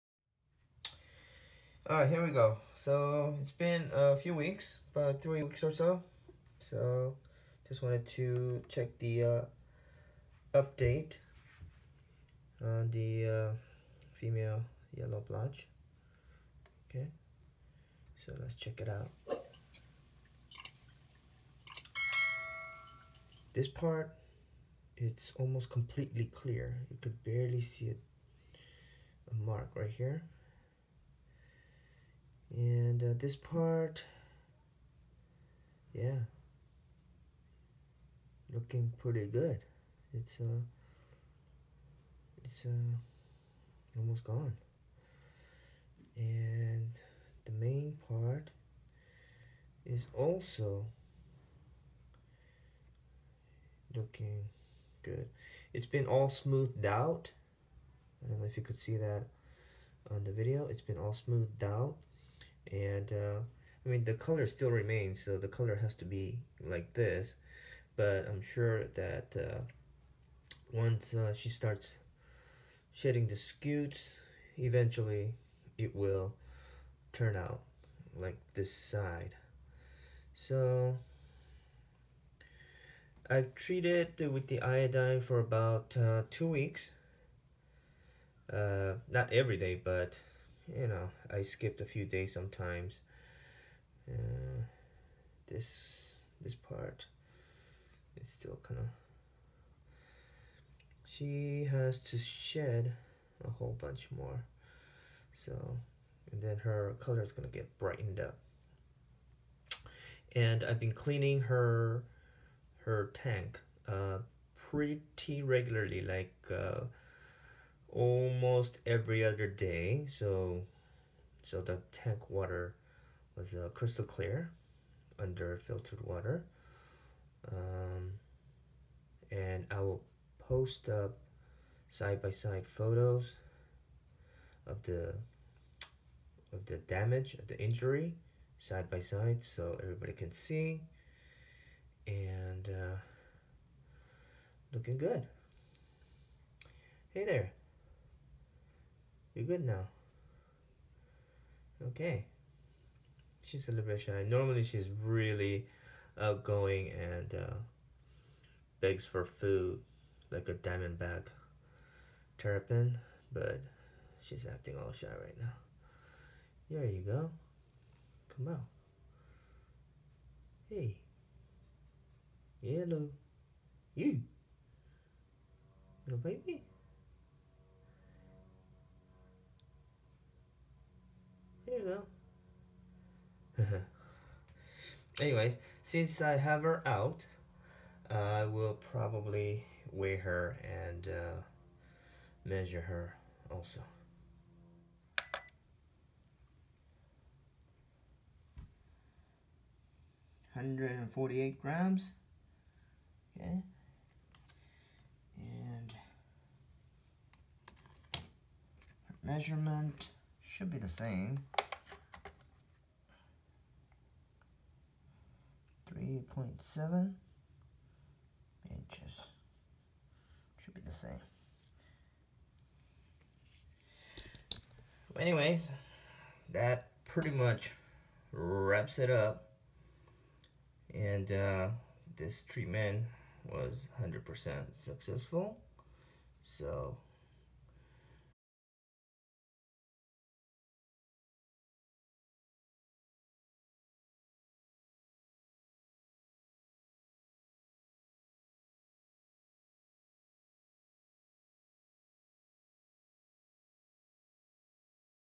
How to treat shell rot and shell damage in turtles | Blotchy shell rot pt2
Shell rot is a very common problem on turtles and tortoises, but more so on water turtles.
On this two part video, I will demonstrate how I treat shell rot and shell damage in my turtles.

0:00 Intro
0:23 inspection
1:22 details on treatment
3:11 getting its weight and measurement
3:57 before and after photos